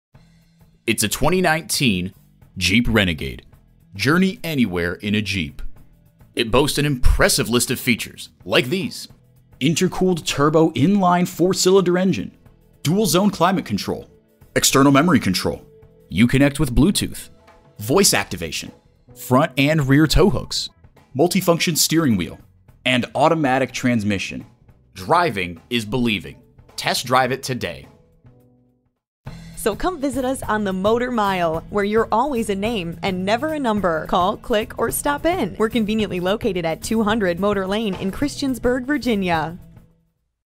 Used 2019 Jeep Renegade Christiansburg VA Blacksburg, VA DC190698 - SOLD
Year: 2019
Make: Jeep
Model: Renegade
Trim: Sport Utility
Engine: 1.3L 4 Cylinder Engine
Transmission: 9-Speed A/T
Color: Black Clearcoat
Interior: Black
Mileage: 43420
Stock #: DC190698
VIN: ZACNJBC18KPJ89915